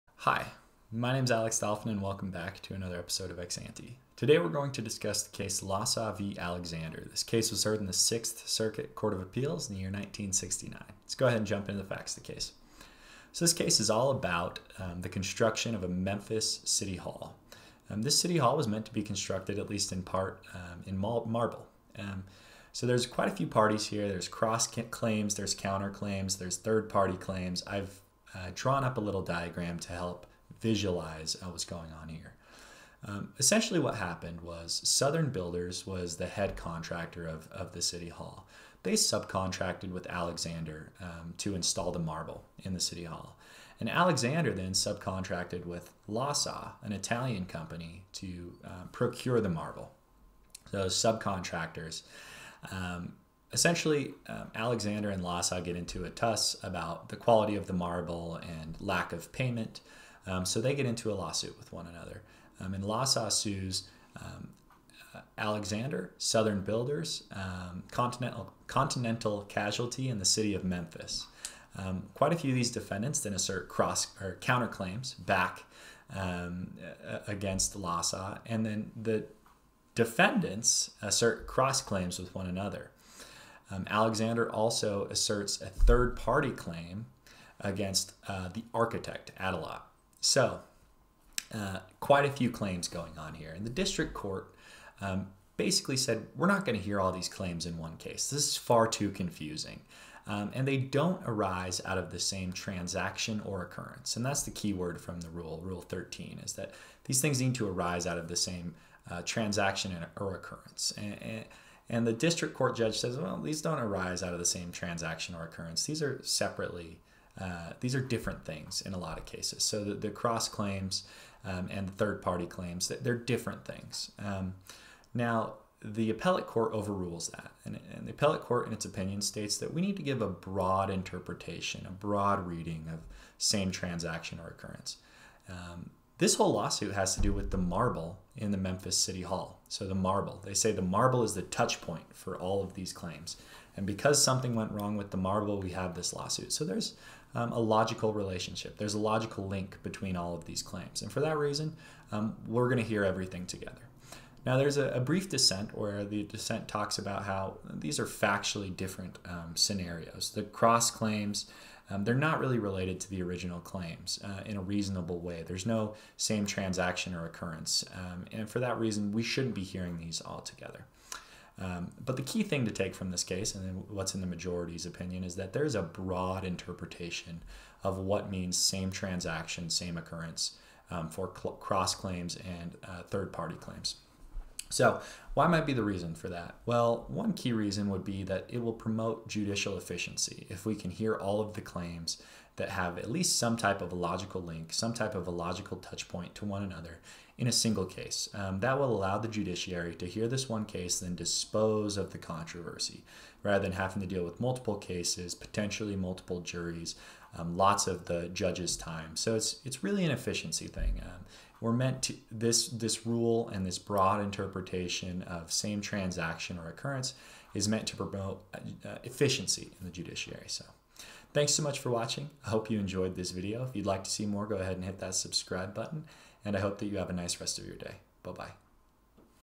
LASA v Alexander | Crossclaims, 3rd Party Claims, & 'same transaction or occurrence'
The 6th Circuit Court of Appeals elaborates on the meaning of 'same transaction or occurrence' for purposes of crossclaims and 3rd party claims.